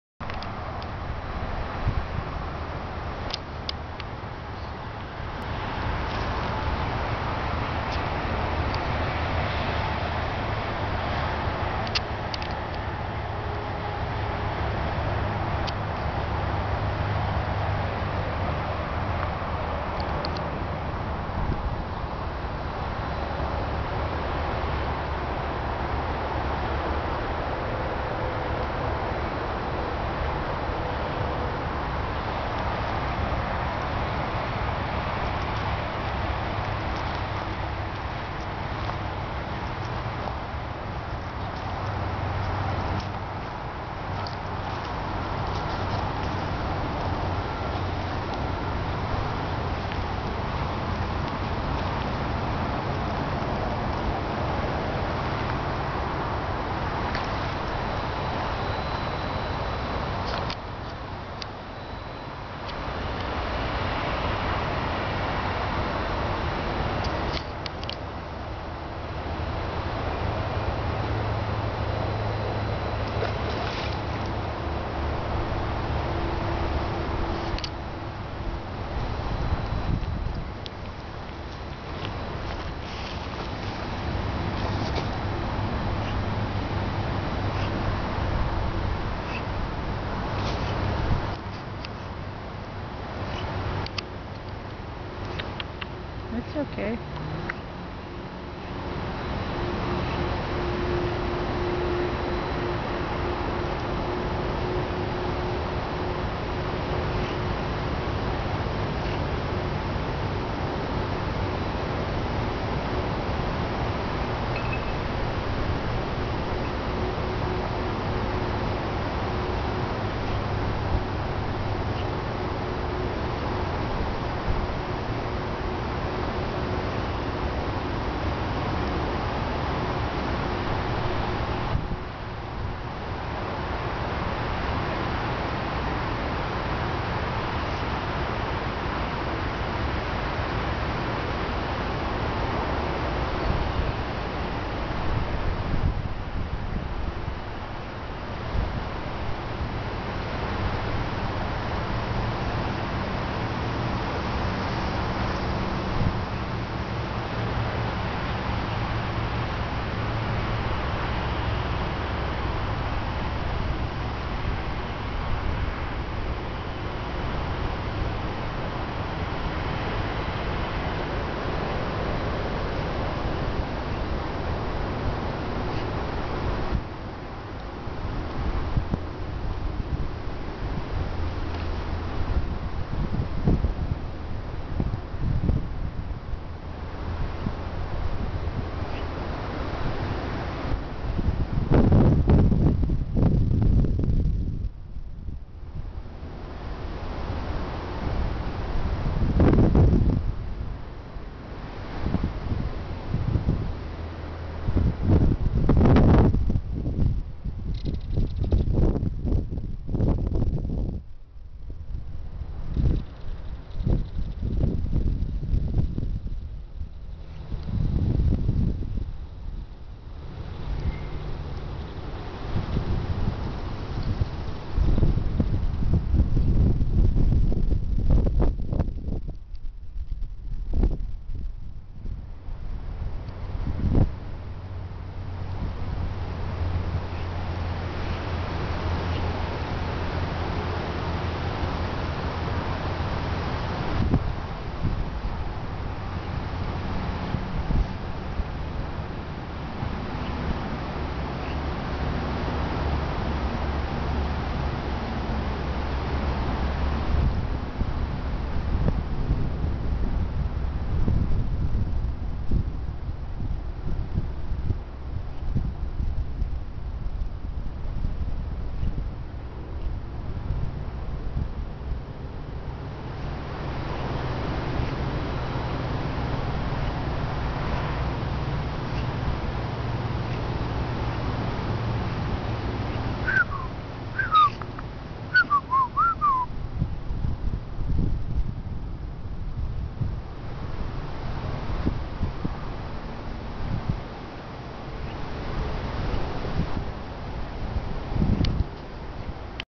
Not The Same Fishing Spot.
beaver ate fish and calls me out, i thought this was my hole ...bugged out already folks